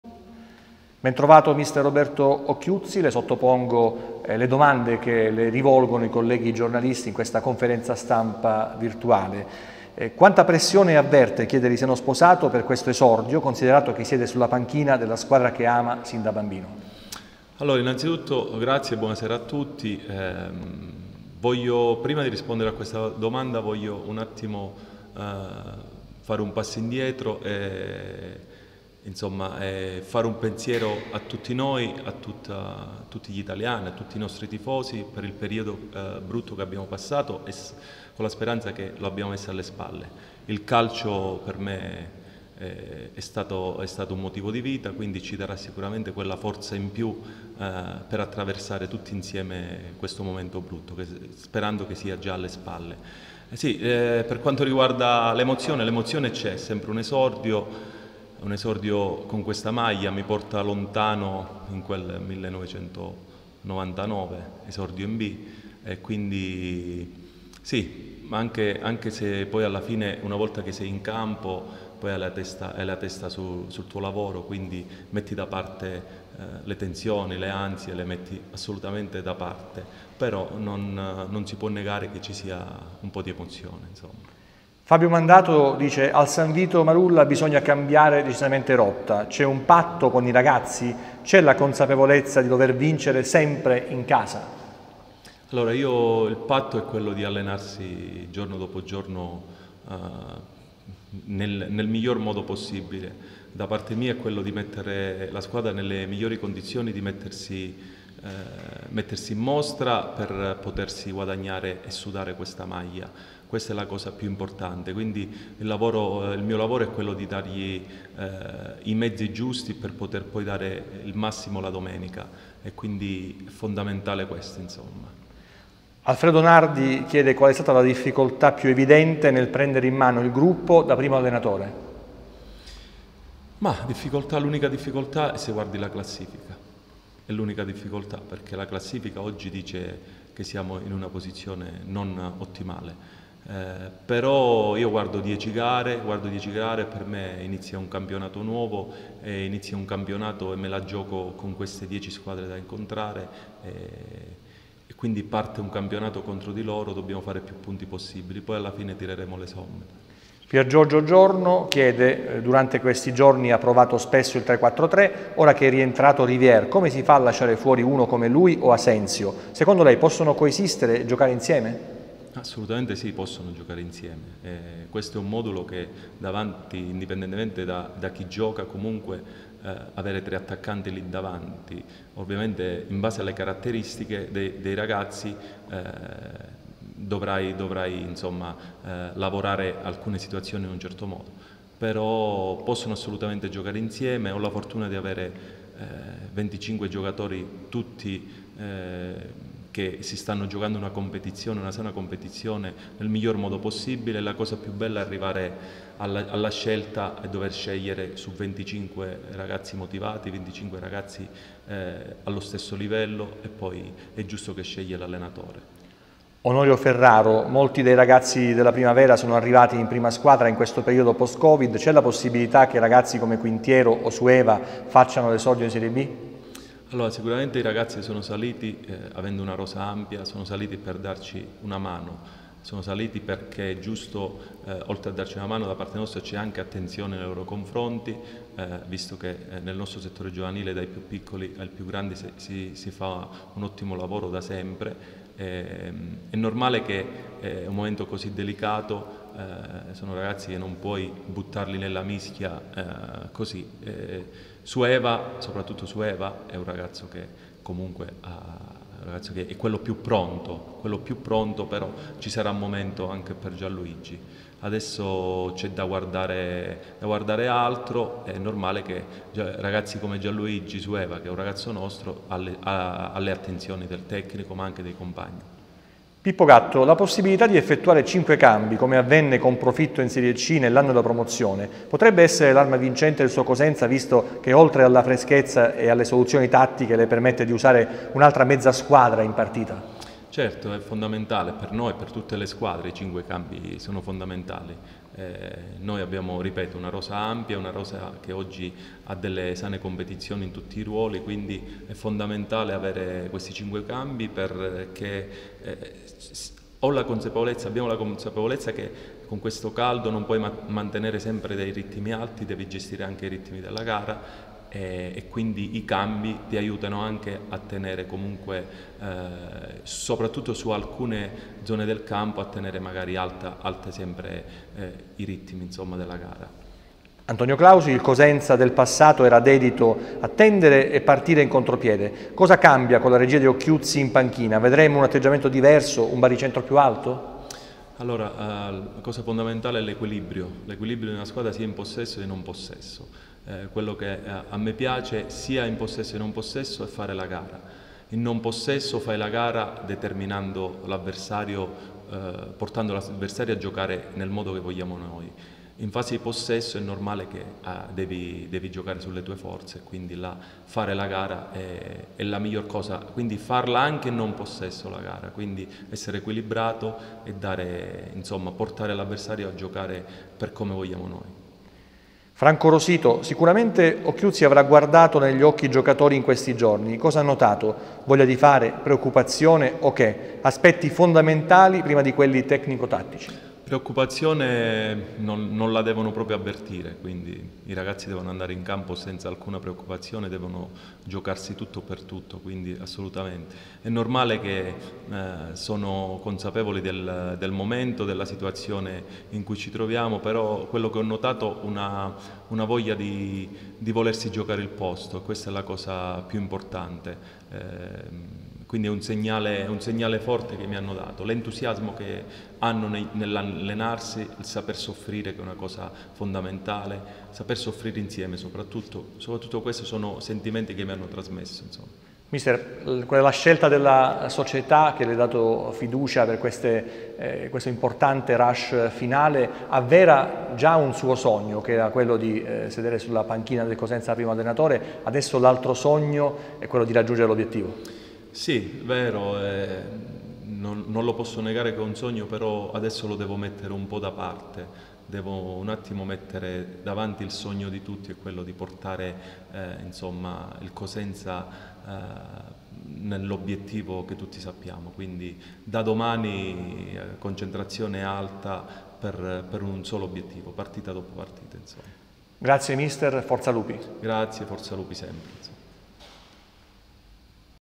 Roberto Occhiuzzi, conferenza stampa 19 giugno 2020
L'allenatore del Cosenza, Roberto Occhiuzzi, presenta la sfida contro la Virtus Entella